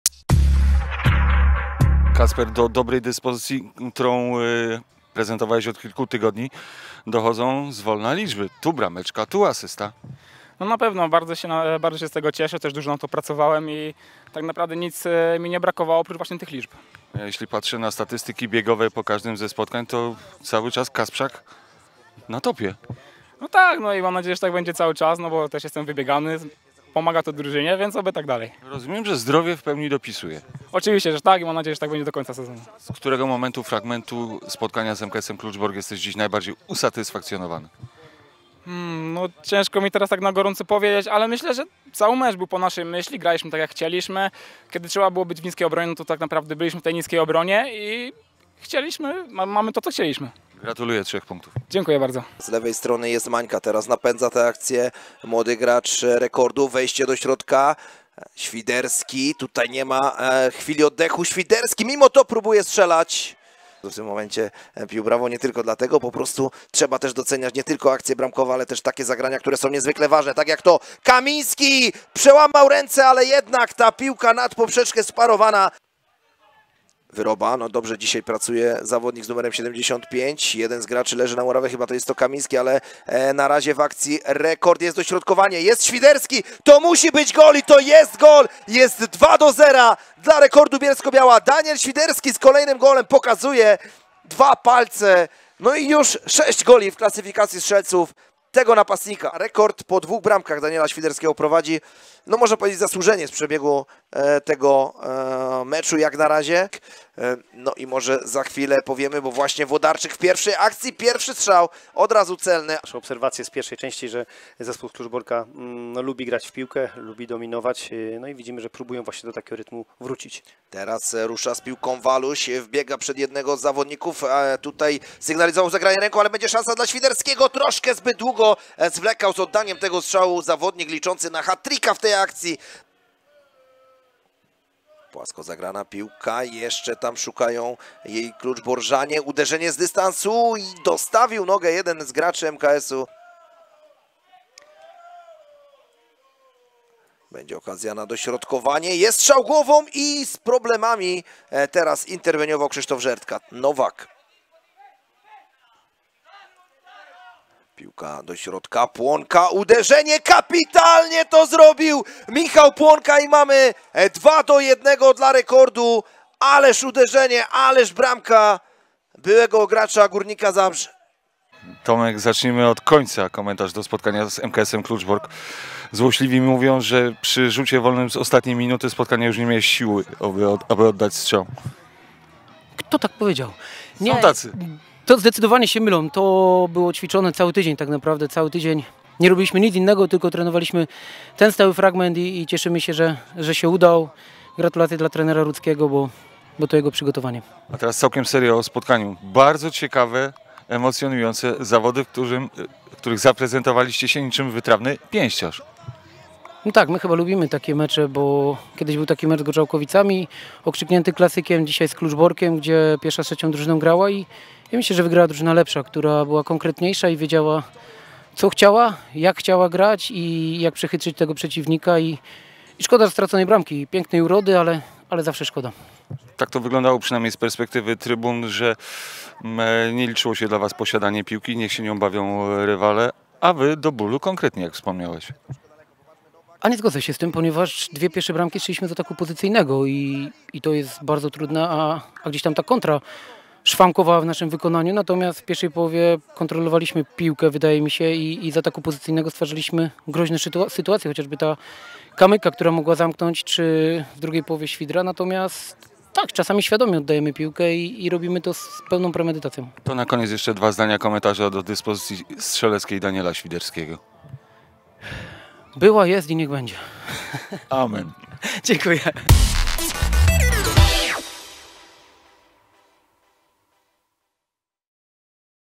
Rekord Bielsko-Biała - MKS Kluczbork 4:1 (2:0)
Trzecioligowy mecz na szczycie, a w nim górą podopieczni Dariusza Klaczy.